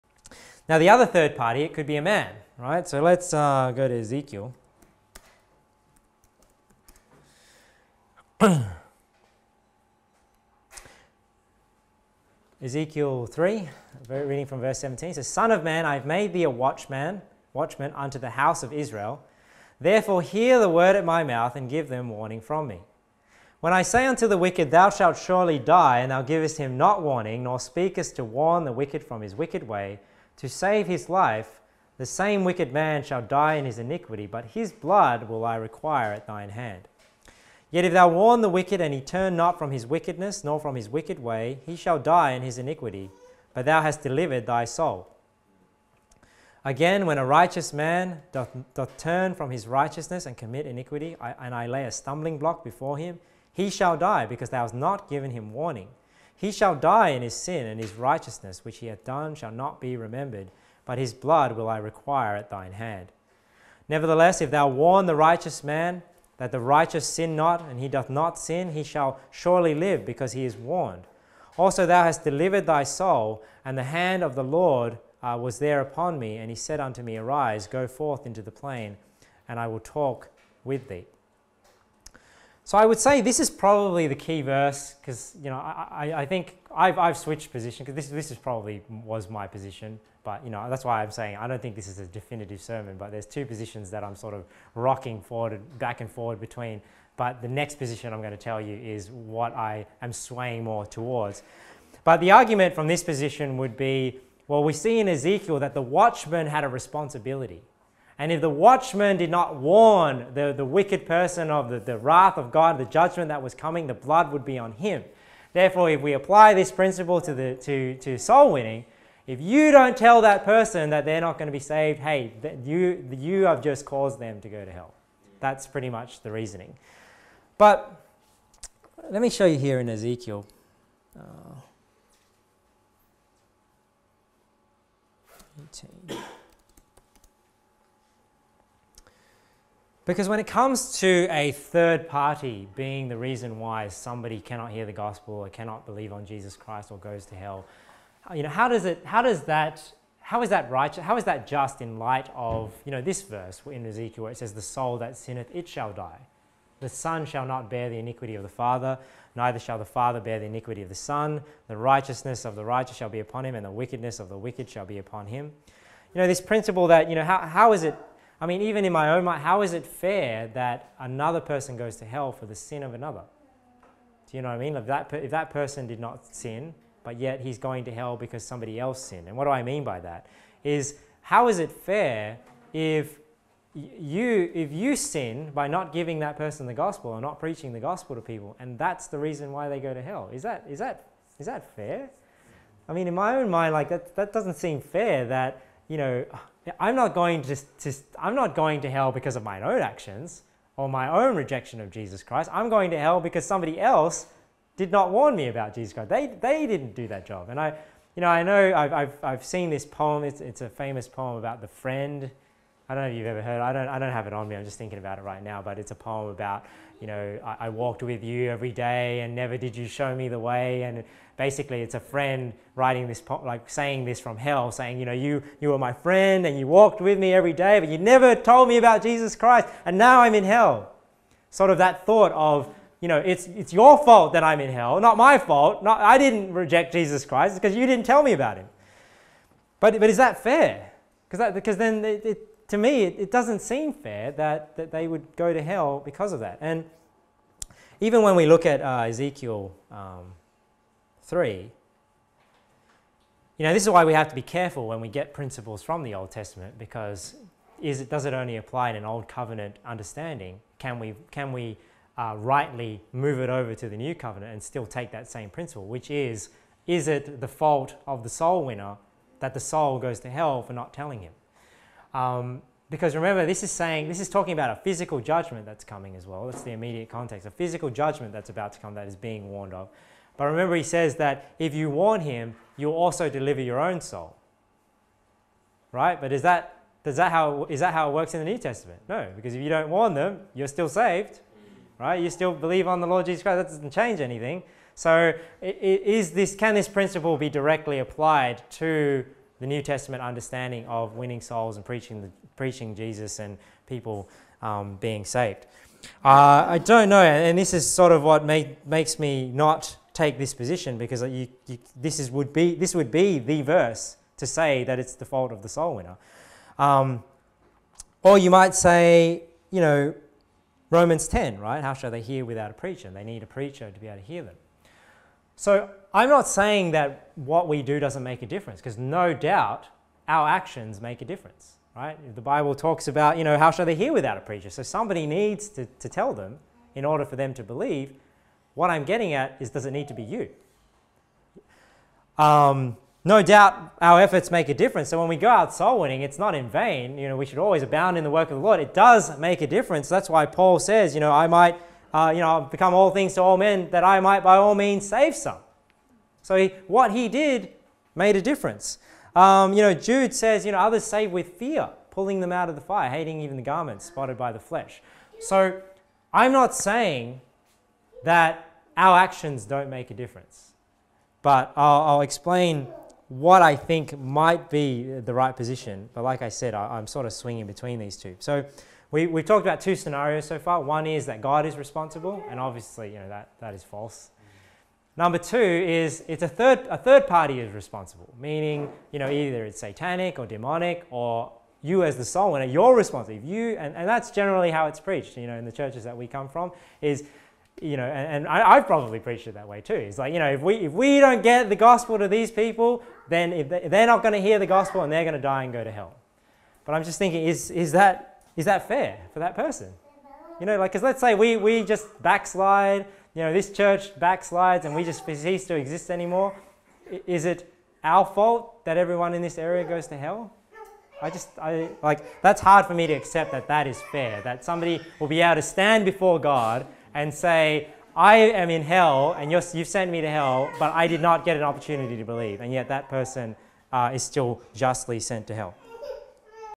Is It Fair for a Soul to Go to Hell Due to My Lack of Soulwinning? (The Watchman, Ezekiel 3:17-21)
This is a clip from the sermon entitled "The Soul That Has Never Heard The Gospel" preached by Victor Tey at The Church in Punchbowl on Sunday, 25th October 2015.